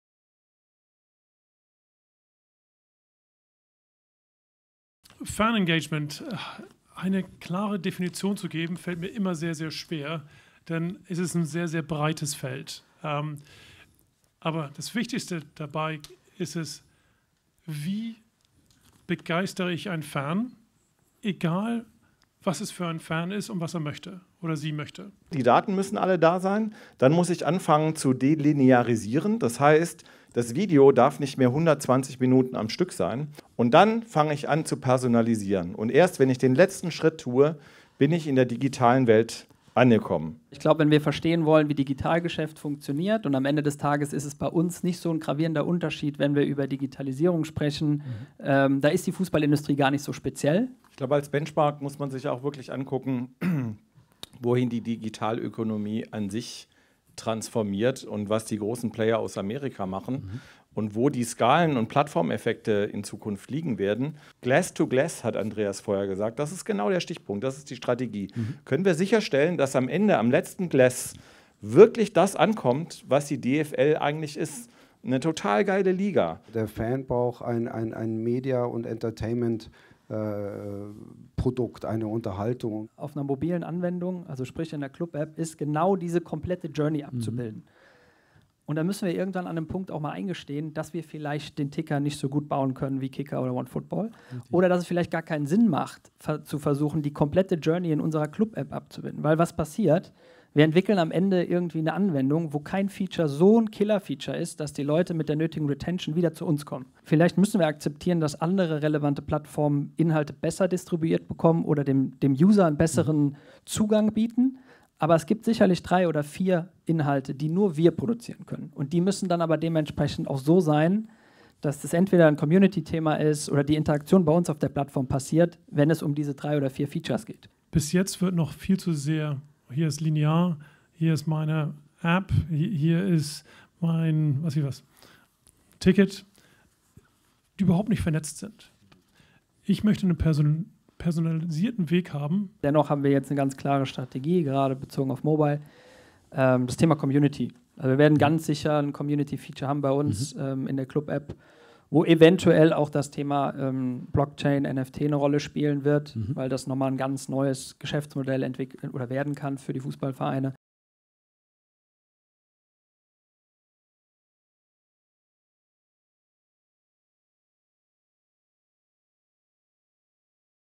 Sky Sport Summit 2021: Fan Engagement
Speakers:

Robert Niemann, Native Waves
Charly Classen, Sky Germany
Tobias Fröhlich, TeraVolt
Philipp Liesenfeld, FC Cologne